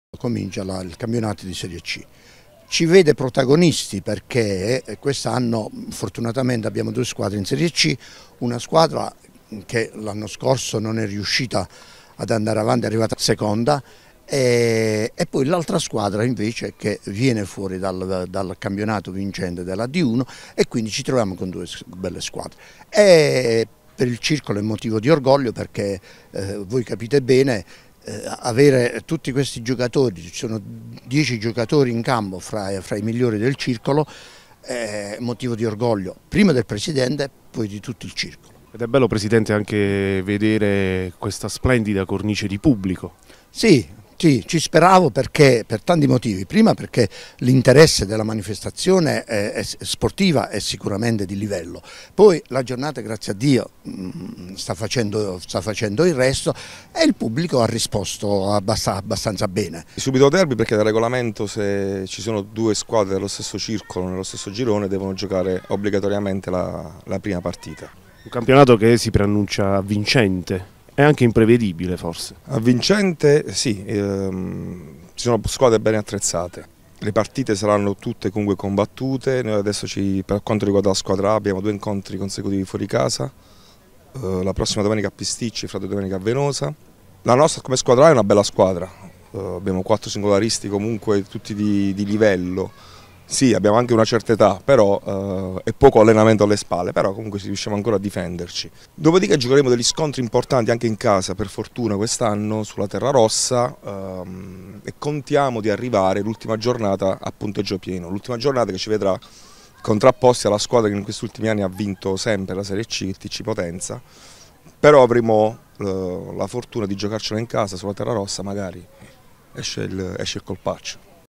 Tutto secondo pronostico la 1° di campionato di Serie C al Circolo Tennis
Ha preso il via il campionato lucano di serie C di tennis maschile. Incontri entusiasmanti sui campi in terra rossa del Circolo Tennis di Matera con di fronte le due compagini locali, la squadra A e la squadra B. Ad imporsi è stata la favorita A (secondo posto nella categoria nella passata stagione) per 4-2 sulla neopromossa B, lo scorso anno vincitrice del campionato di D1.